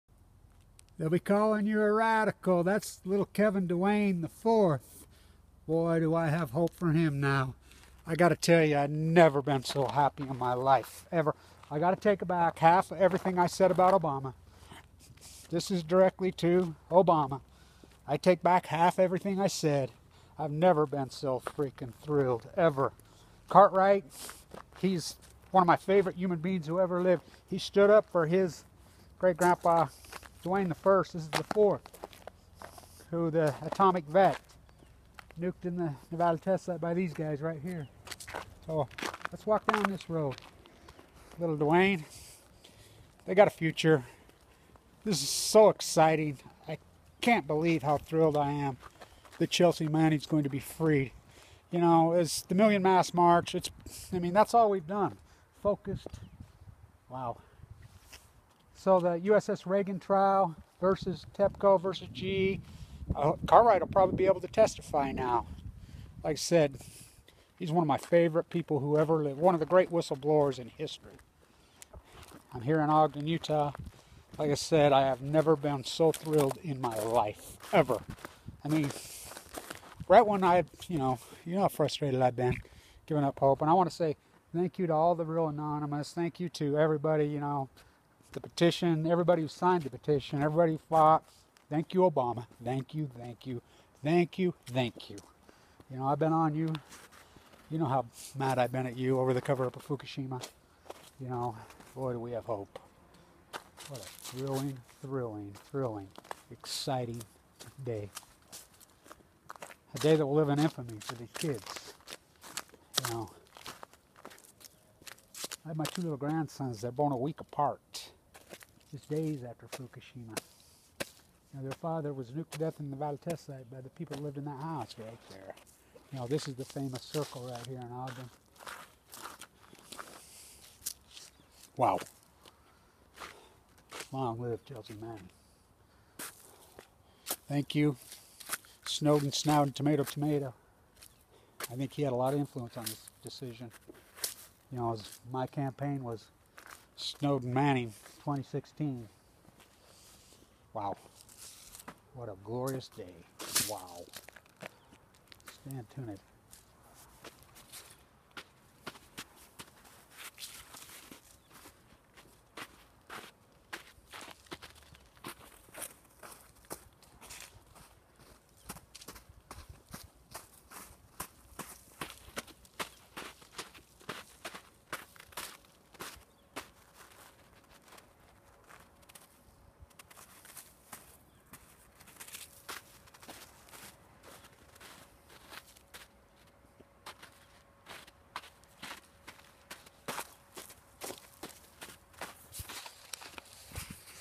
Fukushima breaking news; whistle blowers Cartwright, MANNING set free
Post Ignorance Snowden Cartwright 2020 Infamy INFAMY;; 17 1 17
A day and moment that will life in true History, thank you Obama wow , I have hope thank you to all anonymous million mask marches , Edward Snowden I have hope for my grand children, Sea you in SAN Diego for the U.S. S. Reagan troops vs. the nuclear energy crime syndicate,, Kevin d. Blanch